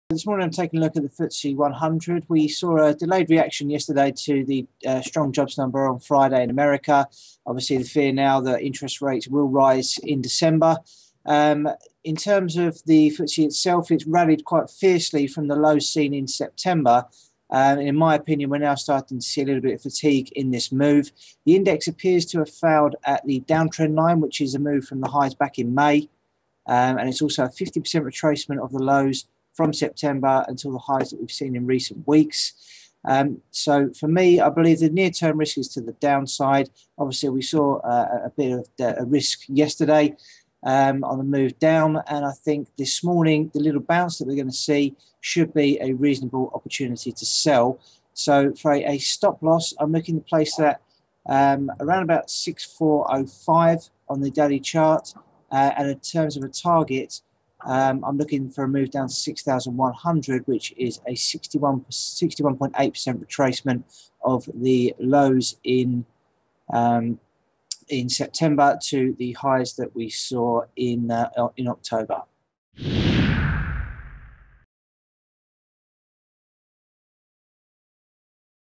Neighbour: FTSE 100 rally running out of steam
The FTSE 100 has seen a strong comeback from the September lows, but
Joe Neighbour says the rally is looking like it is running out steam
and is looking to short the UK index.
According to Neighbour, the index appears to have failed at the
downtrend line on the daily chart, which dates back to the highs in
May and has capped any upside advances since. The FTSE 100 has almost
retraced 50% of the large down move earlier this year, and Neighbour
believes that in the near term there is a risk to the downside.
He is looking to sell into any strength and would ideally like to sell
at 6330 and target a move towards 6100, a 61.8% retracement of the
recent up move. His stop loss on the trade is 6405.